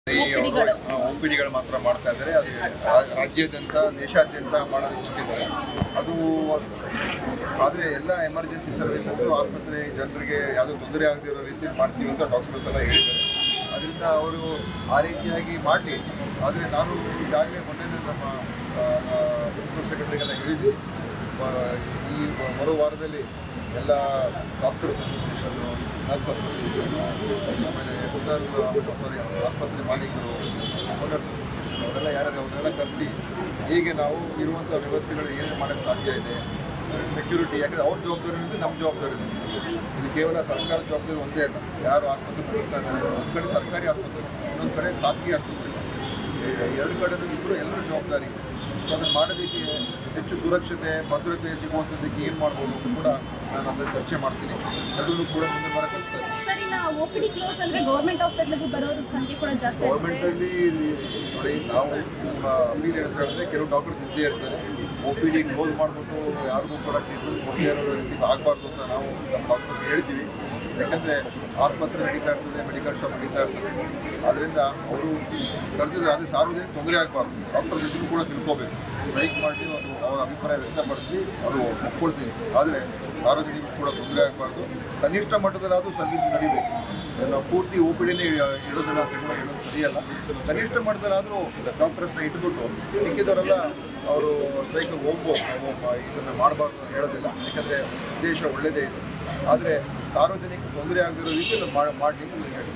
Dinesh Gundu Rao Speaks on Doctors' Protest | ‘ಸ್ಟ್ರೈಕ್ ಮಾಡಲಿ ಆದ್ರೆ ಸಾರ್ವಜನಿಕರಿಗೆ ತೊಂದರೆ ಆಗಬಾರದು’
‘ಸ್ಟ್ರೈಕ್ ಮಾಡಲಿ ತೊಂದರೆ ಇಲ್ಲ ಆದ್ರೆ ಸಾರ್ವಜನಿಕರಿಗೆ ತೊಂದರೆ ಆಗಬಾರದು’ -  ಸಚಿವ ದಿನೇಶ್ ಗುಂಡೂರಾವ್